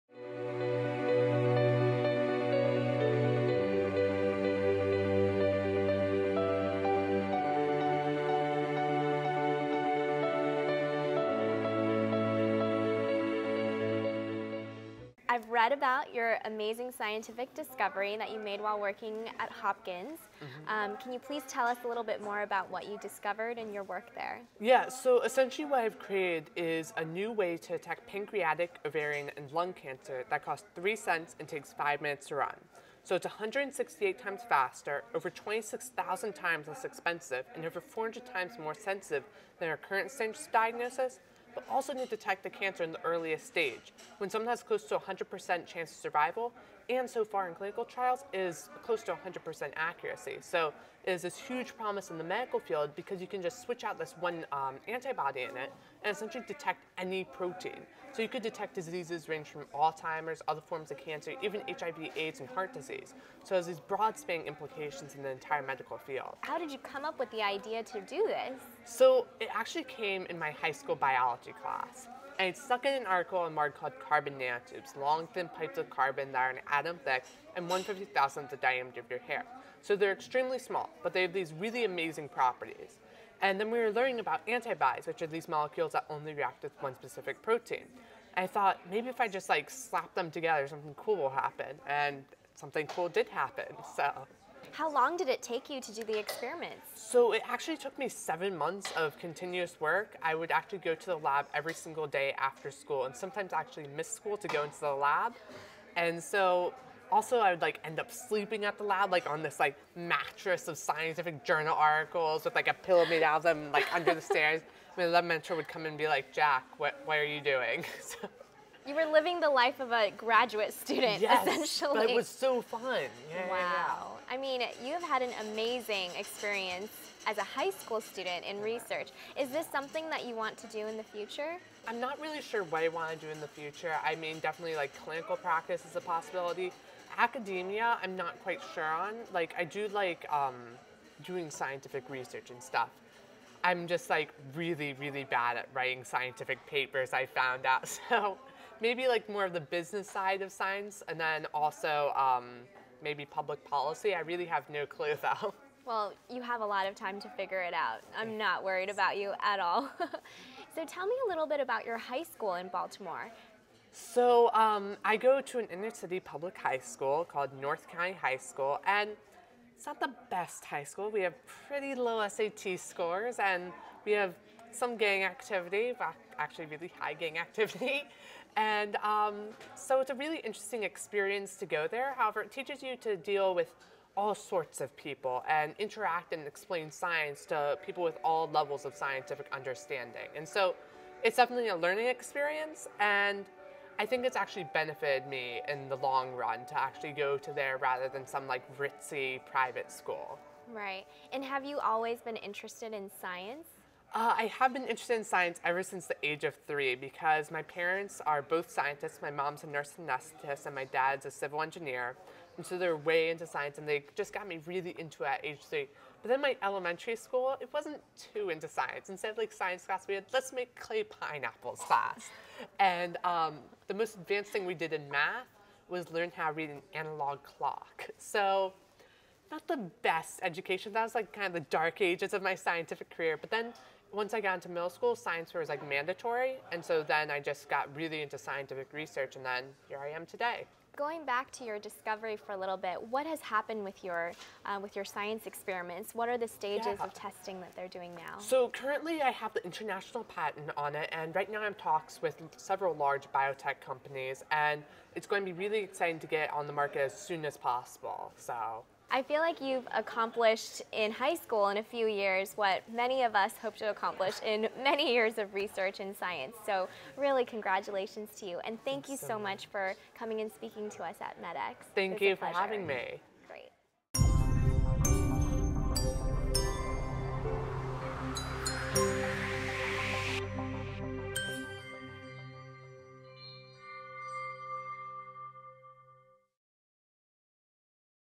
Jack Andraka - Stanford Medicine X 2013 - Joyce Ho in Conversation with Jack Andraka
Stanford Medicine X host Joyce Ho in conversation with Jack Andraka on research and open access